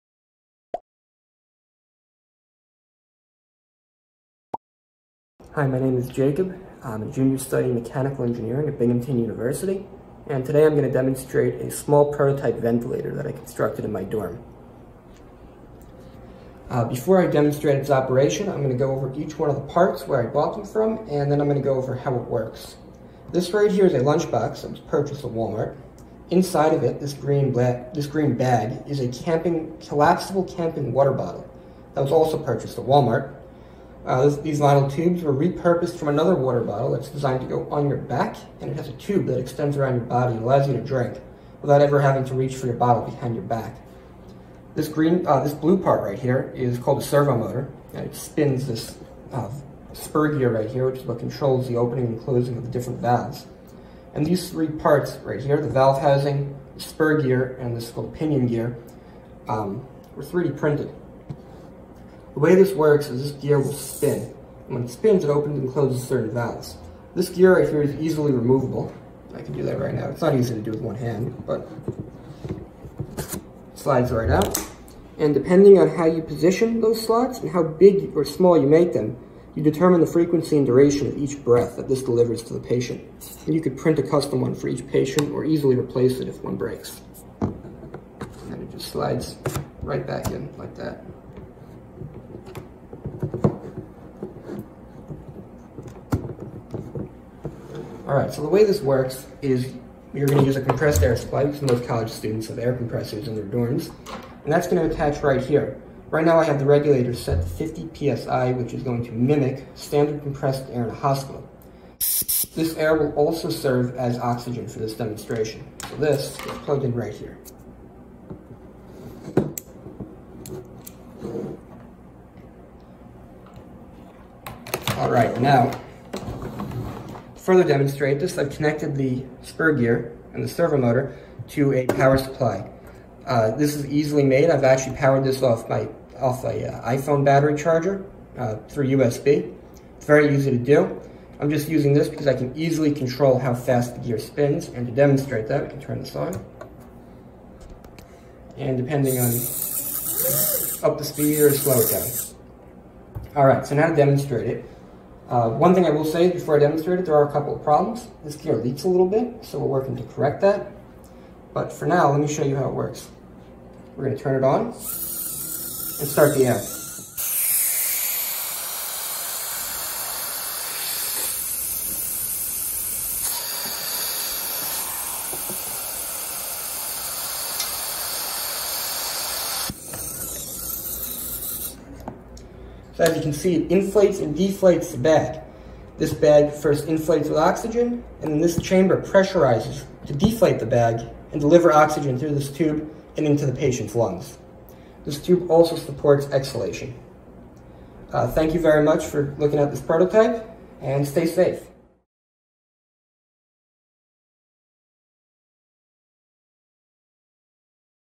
Binghamton University Student Builds Ventilator Prototype to Assist During COVID-19 Pandemic
Jacob Goodman, a mechanical engineering student at Binghamton University, State University of New York, built a ventilator prototype using mainly parts he purchased from Walmart, to help during the coronavirus pandemic. Here he breaks down the parts and how the device works.